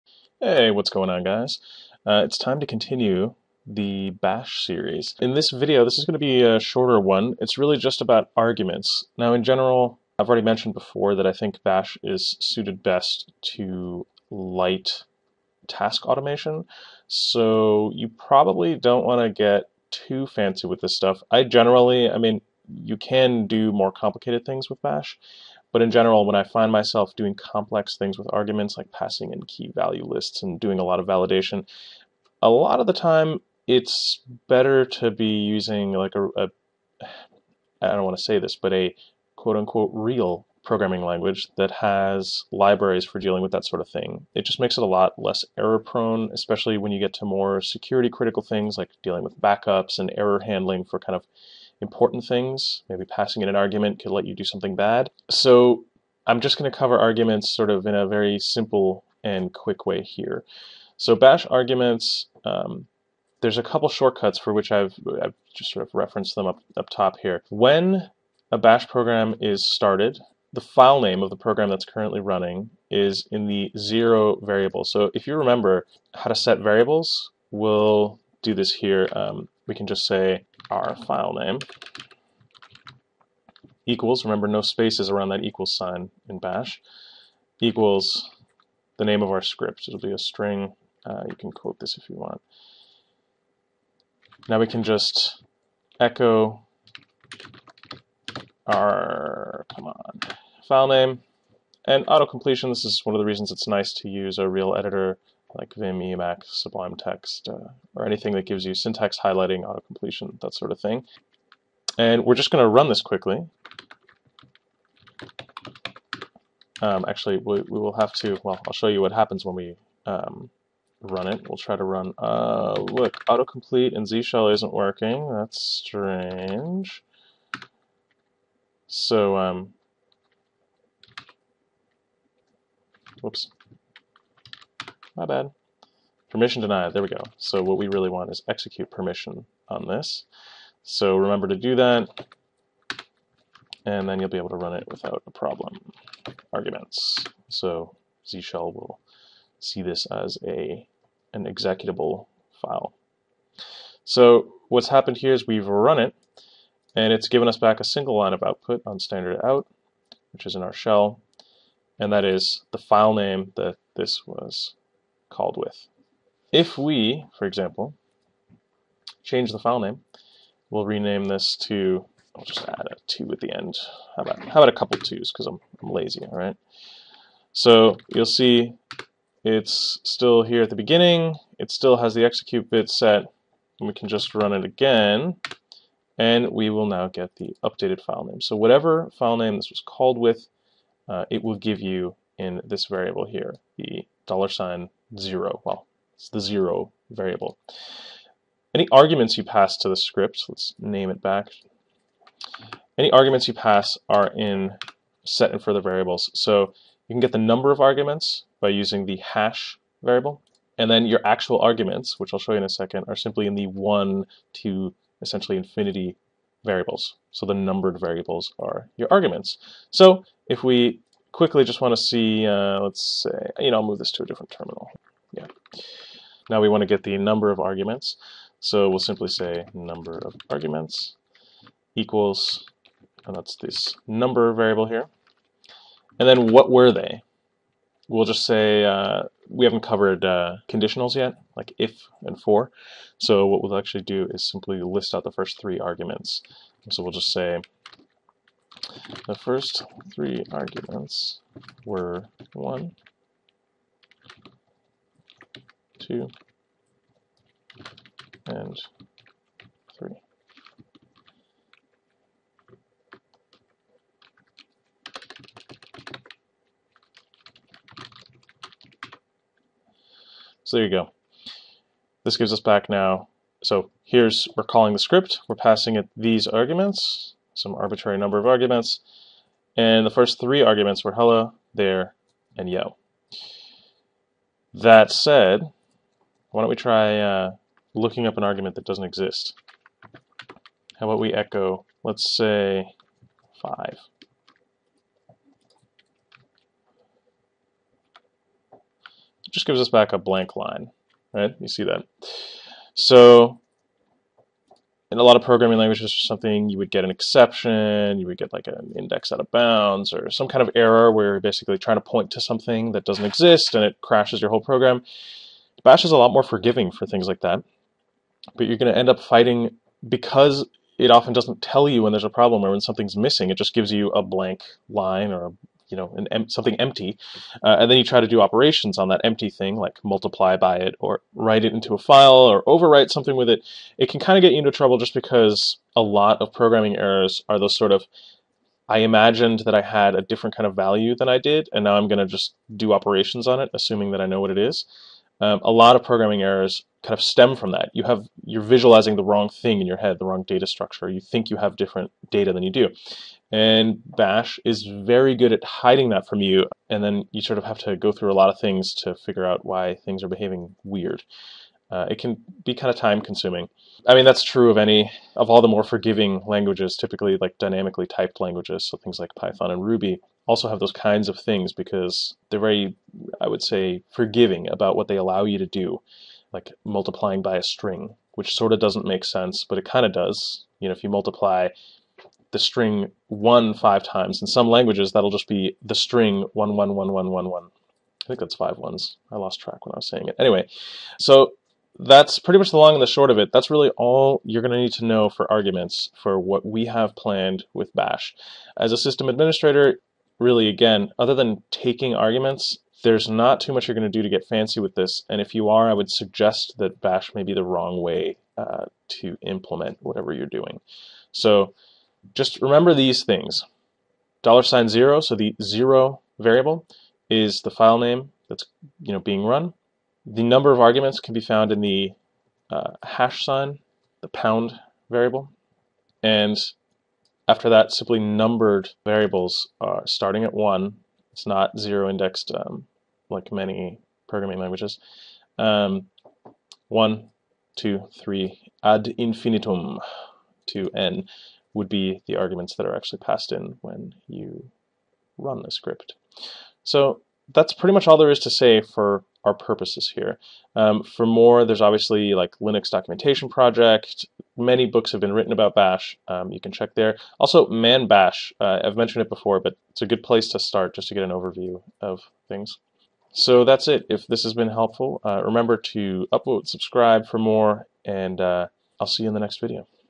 Bash Scripting 5 -- Arguments
How do arguments work in Bash scripting? How can my bash script accept input? Here's a quick video covering the basics of arguments in Bash scripting. Arguments are the data that you pass in when you call a script, like "scriptname argument1 argument 2".

In real life, this is something like 'tar zcf filename.tar.gz directory' ('zcf', 'filename.tar.gz', and 'directory' are the arguments in this example).

$# -- number of args that our script was run with
$0 -- the filename of our script
$1..$n -- script arguments